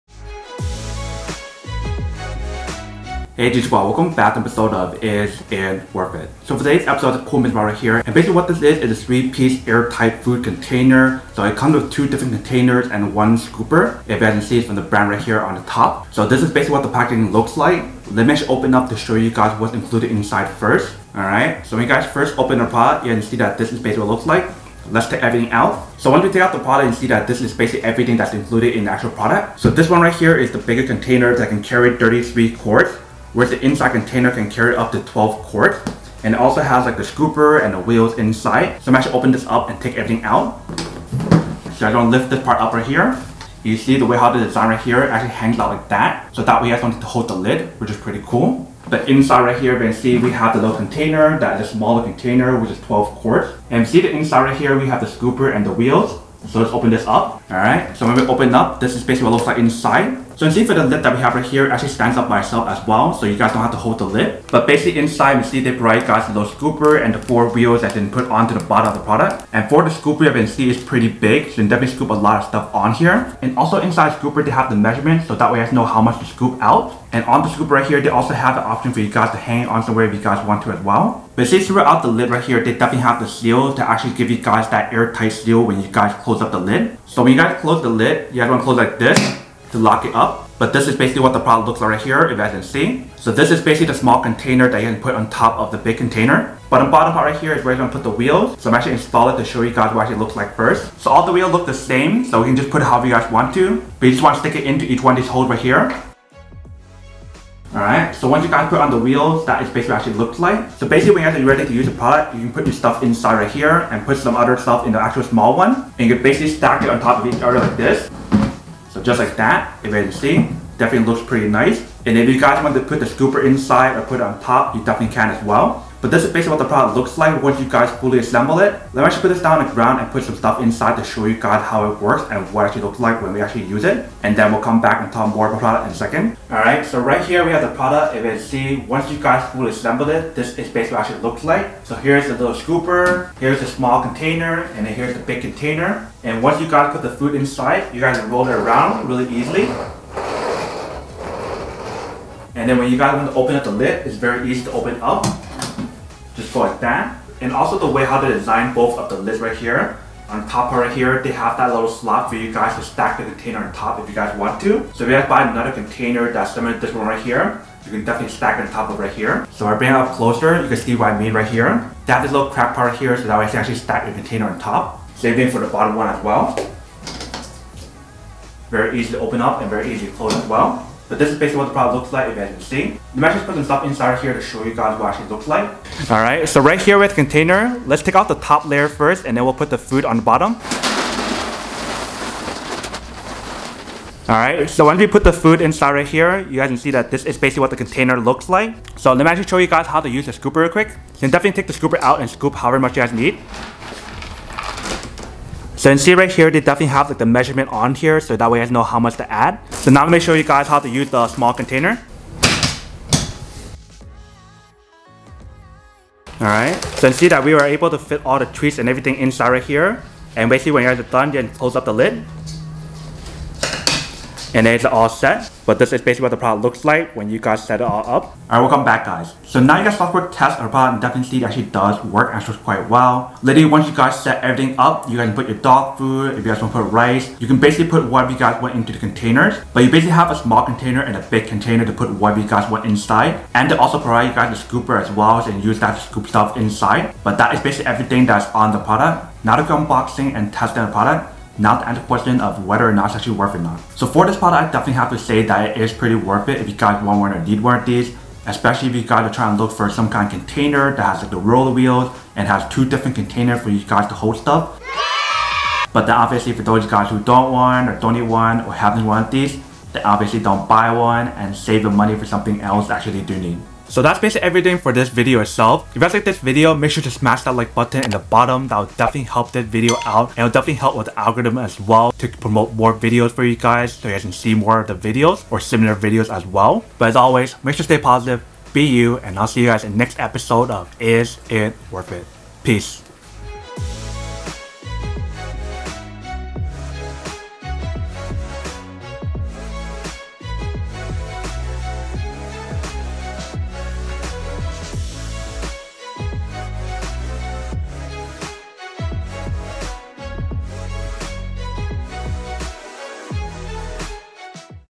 How to Use IRIS USA Airtight Food Storage Container?
Are you in search of the best container to store your food for your pet or food in general? Check out the video to find out the solution!

If you want:
IRIS USA 3-Piece Airtight Food Storage Container Combo